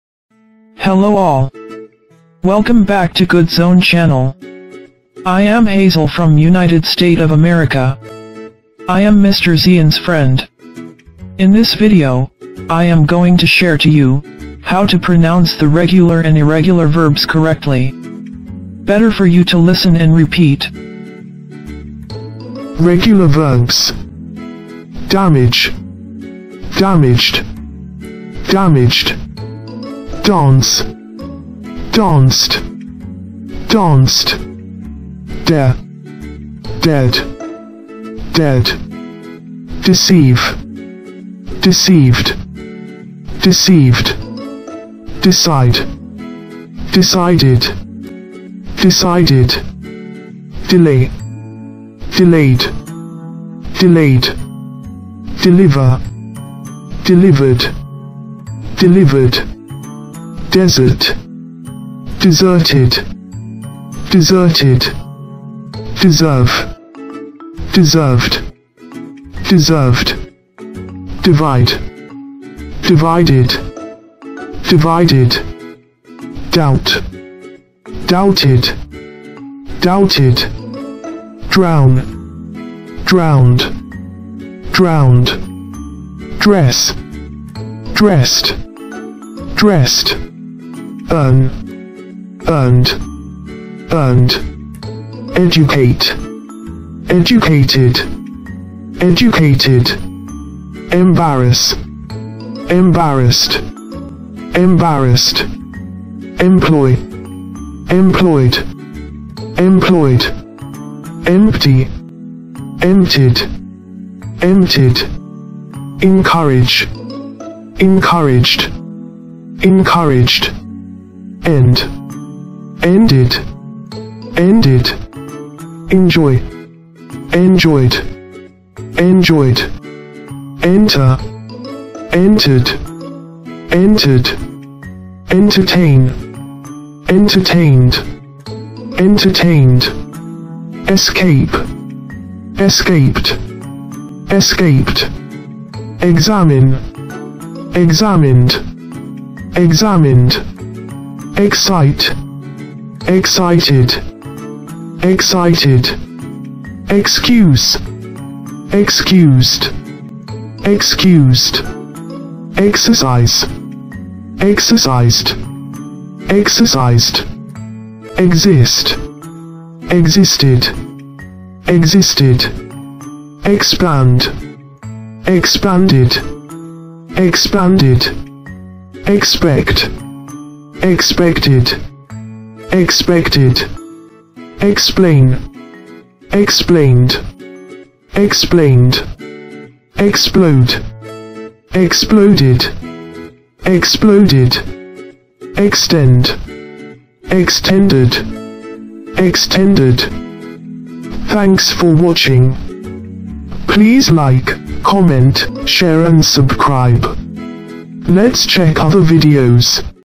A-Z REGULAR VERBS dan CARA MEMBACA SESUAI NATIVE SPEAKER - PART 3
Hello semuanya.
Video ini daftar lengkap Regular Verb beserta cara membacanya
sesuai dengan Native speaker.